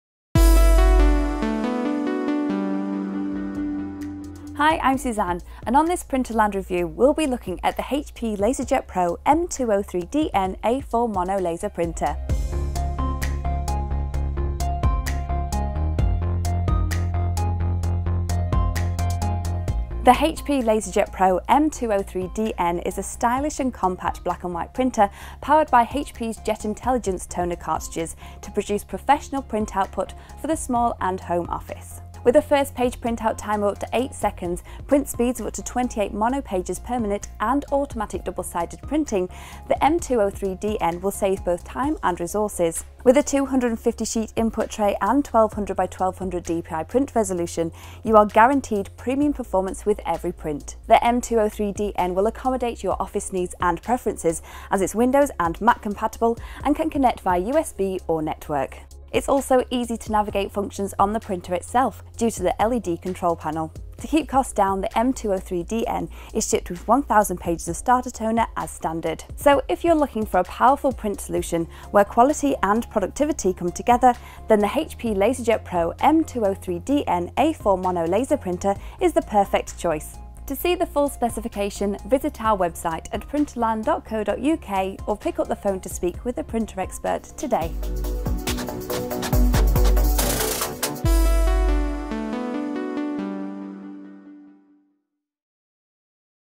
HP LaserJet Pro M203dn Printer | Product Overview
Description:Get more pages, performance, and protection1 from an HP LaserJet Pro powered by JetIntelligence Toner cartridges. Set a faster pace for your business: Print two-sided documents right away, and easily manage to help maximize efficiency. Features Fast printing, low energy use Print at the pace of business—this printer is equipped with fast two-sided printing. Grab the documents you need and go. Print first pages in as fast as 6.7 seconds.5 Help save energy with HP Auto-On/Auto-Off Technology. Manage company-wide printing with confidence Centralize control of your printing environment with HP Web Jetadmin—and help build business efficiency.8 Identify new devices and gain insight into printing with HP Web Jetadmin remote discovery and monitoring